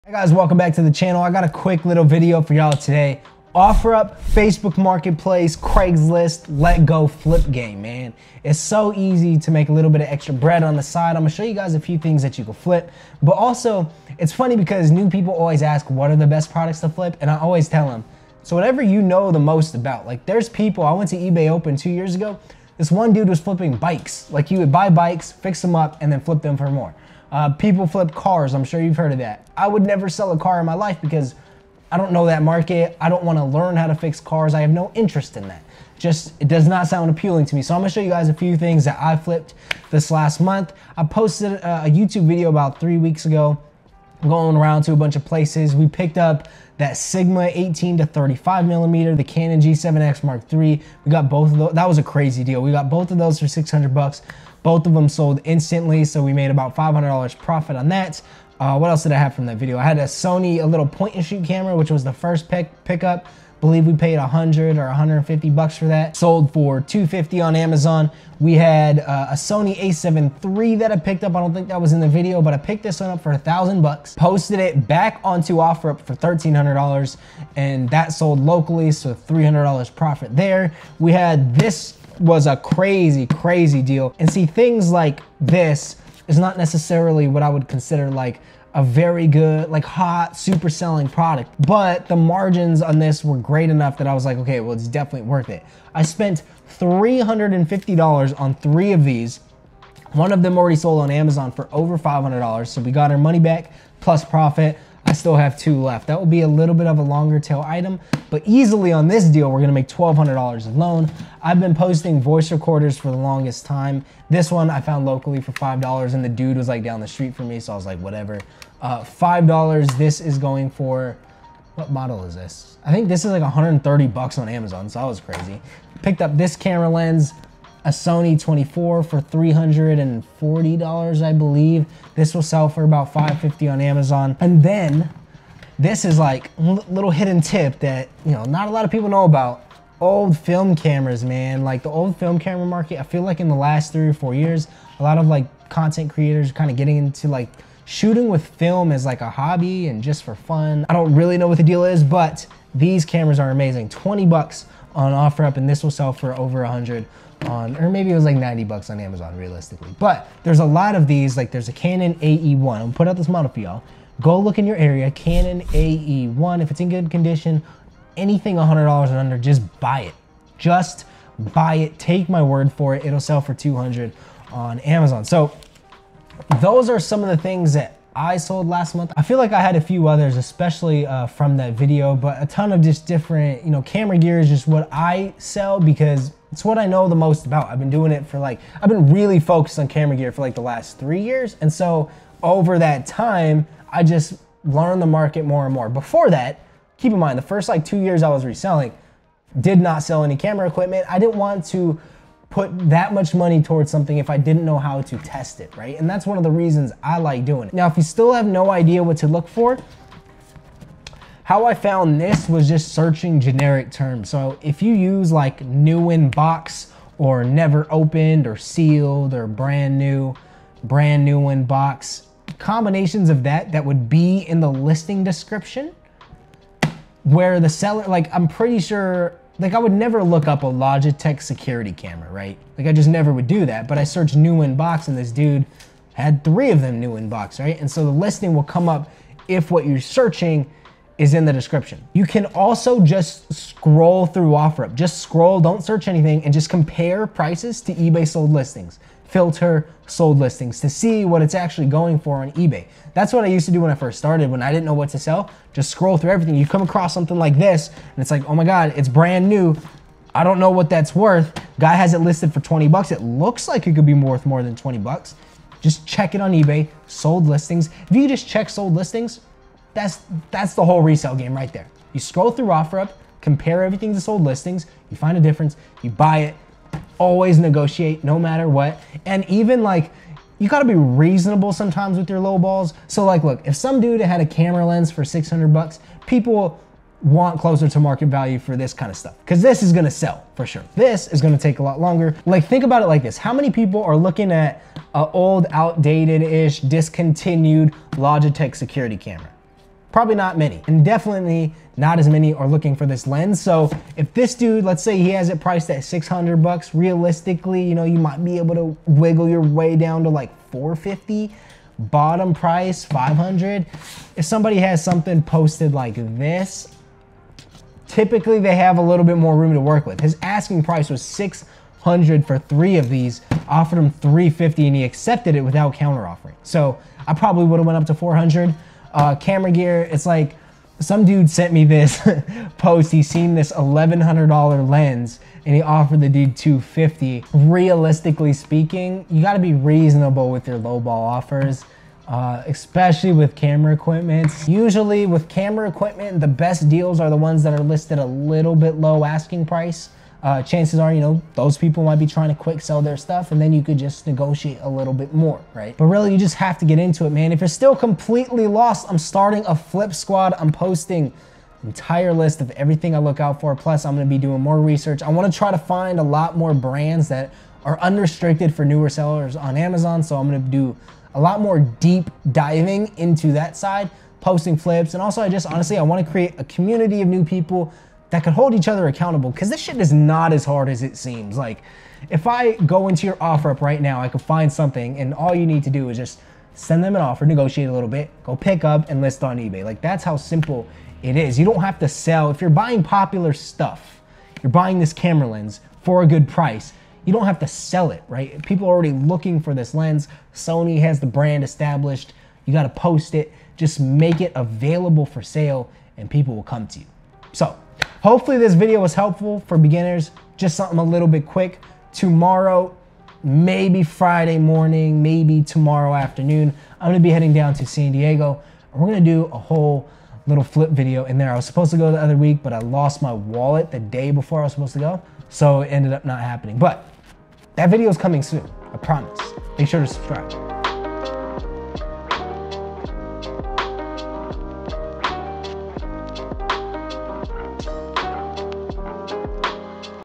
Best Products to Resell on Amazon FBA in 2022
Still the absolute best side hustle in the world. Keywords: SIDE HUSTLE. Not full on hustle. I'm talking about you could literally be spending your 15 min break at work looking for things to flip, pick them up after work, post them online, and make extra cash on the side with ease.